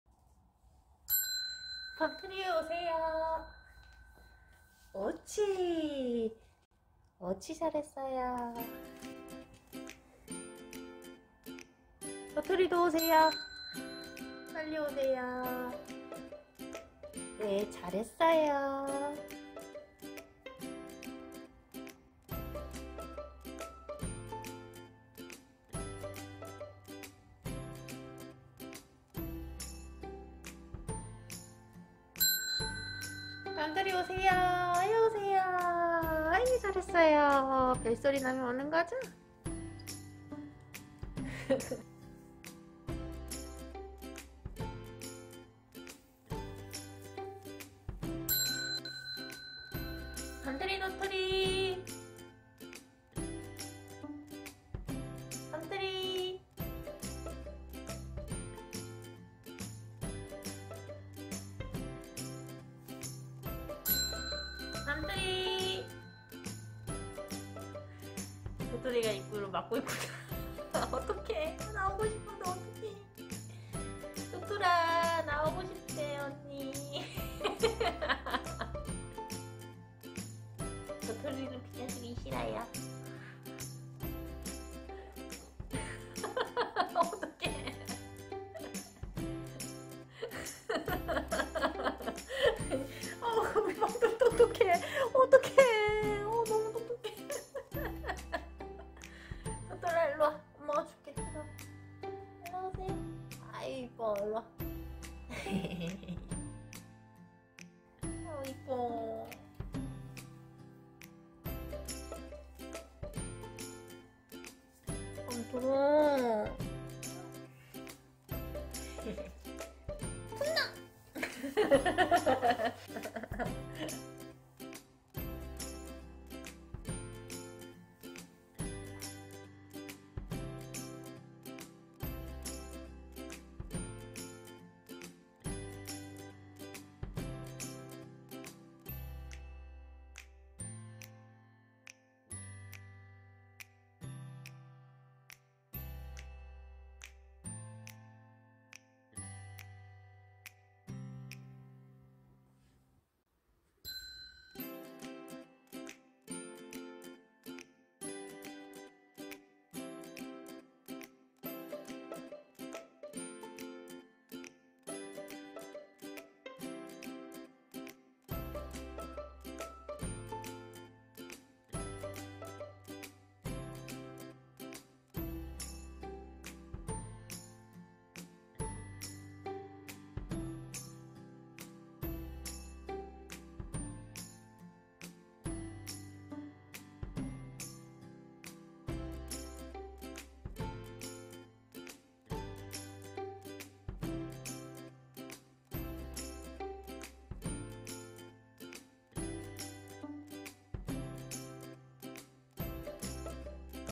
Guinea Pig Bell Education 반려동물 기니피그 종 훈련 モルモットベルトレーニング 강형욱 V
밤톨맘 동생이 비타민을 주면서 벨 교육을 했어요

Give me your favorite snack when you hear the ringtone
着信音が聞こえたら、好きなおやつをください

아티스트
Benjamin Tissot